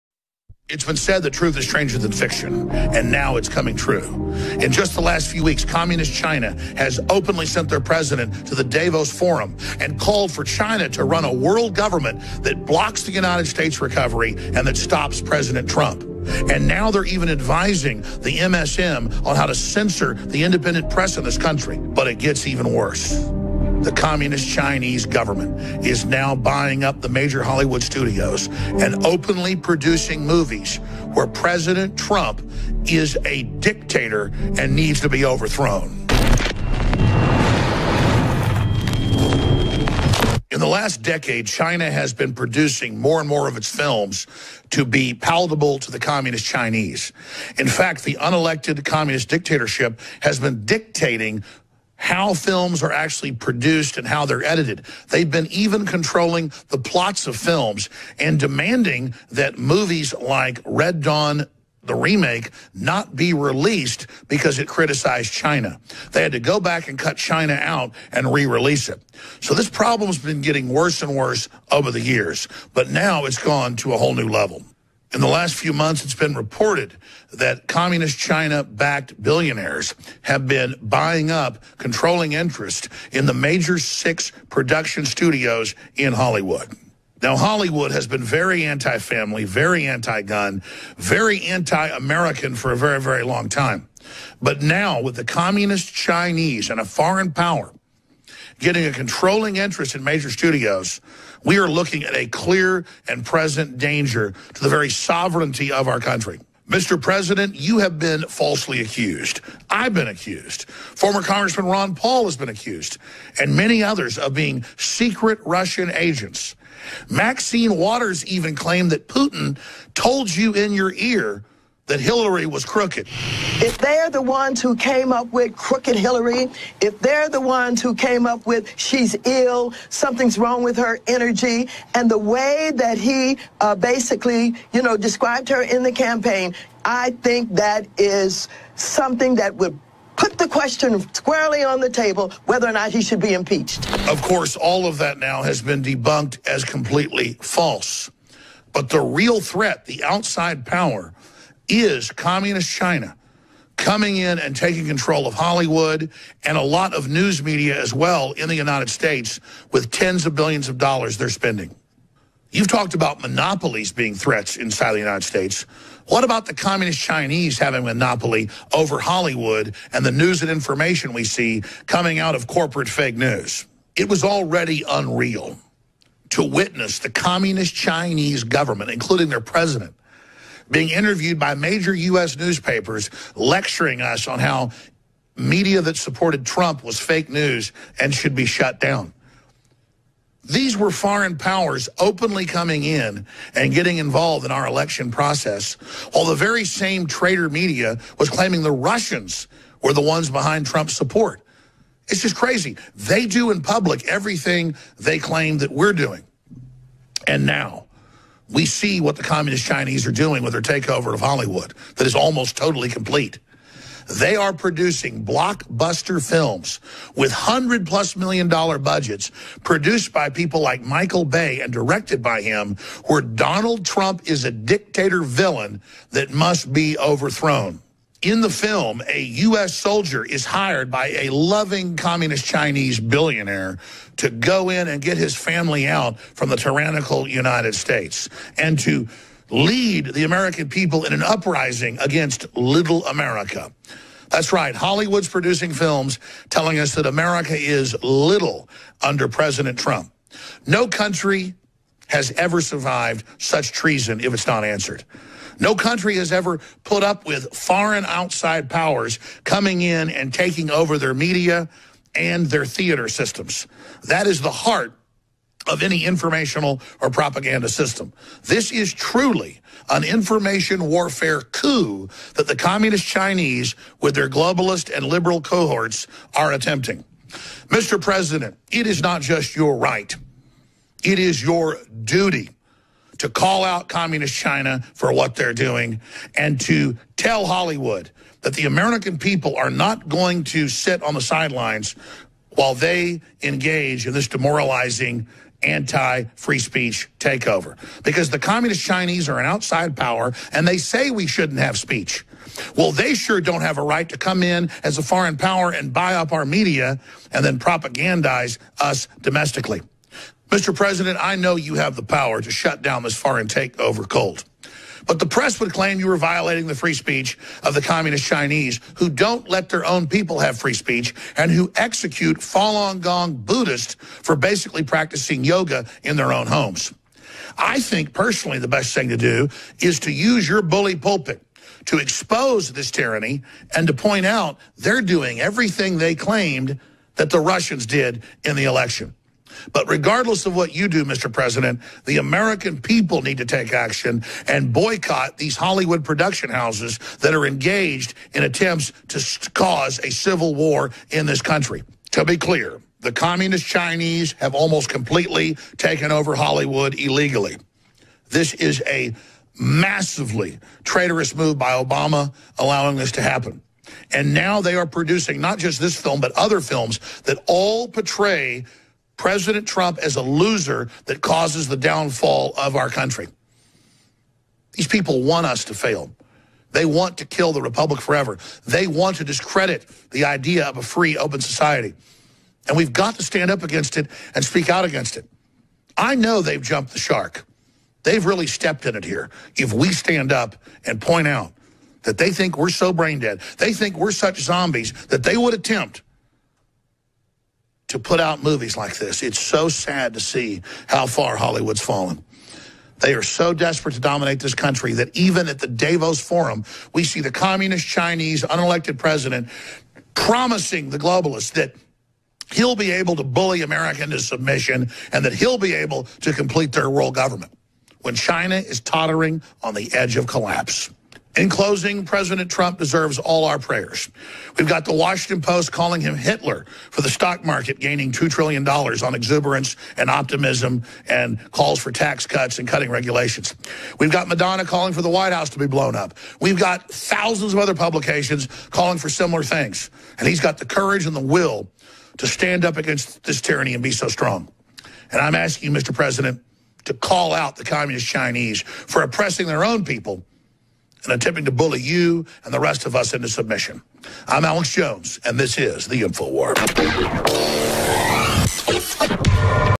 CHINA BUYS 6 HOLLYWOOD STUDIOS!! Alex Jones Exposes Chinese Propaganda! Buying America
Chinese are buying Hollywood!